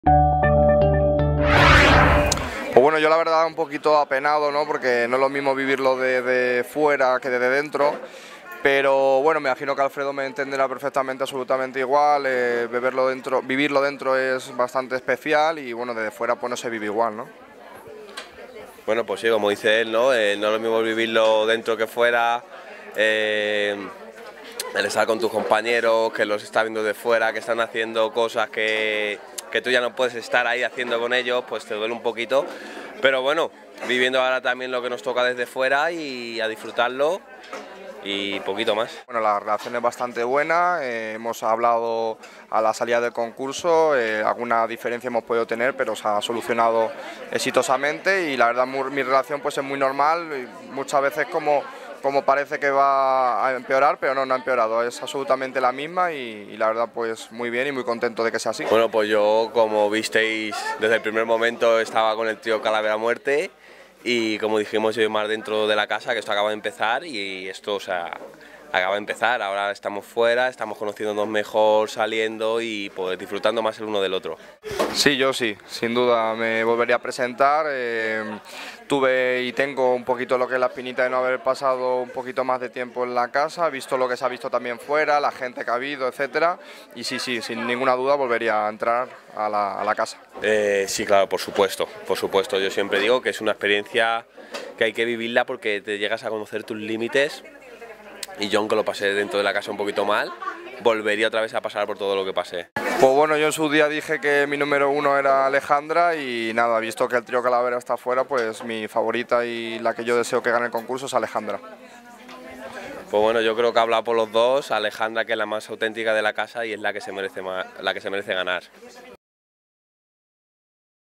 Alfredo ('GH15'): "Fuera del concurso estoy conociendo mejor a Omar"
Entrevistamos a los exconcursantes de 'Gran Hermano 15' para que no cuenten como están viviendo el concurso ahora que están fuera de la casa de Guadalix. Vitín, que fue eliminado en la séptima gala del concurso, habla de su relación con su mejor amigo Omar. Los concursantes nos descubren además sus favoritos para ganar el reality ¿Quiénes serán?